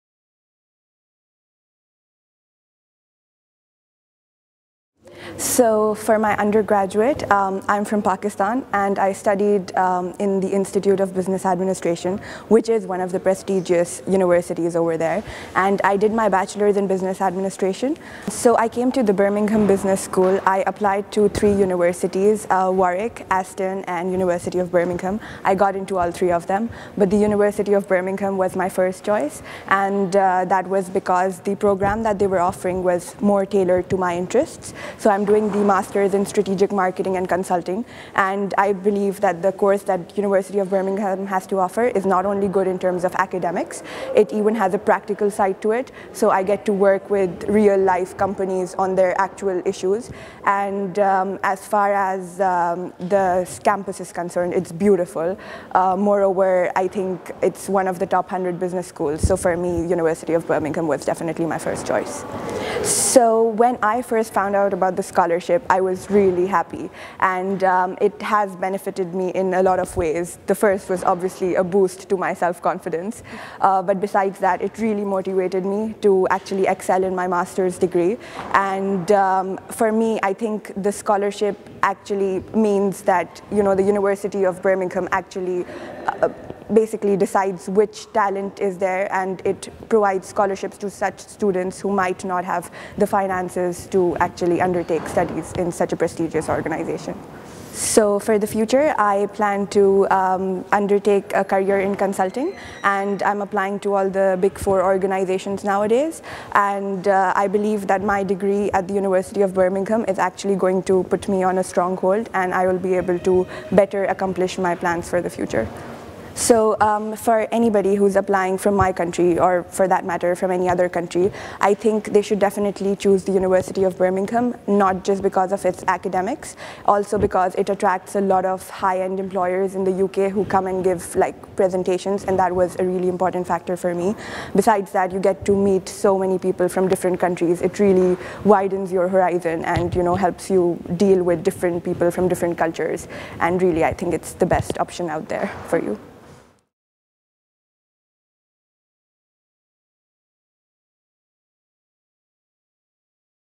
Meet our MSc Business Scholars: Farah Siddiqui
Recipients of our MSc Business Scholarships talk about why they chose Birmingham Business School, their impressions so far, the difference the scholarship has made to their studies and their ambitions for the future.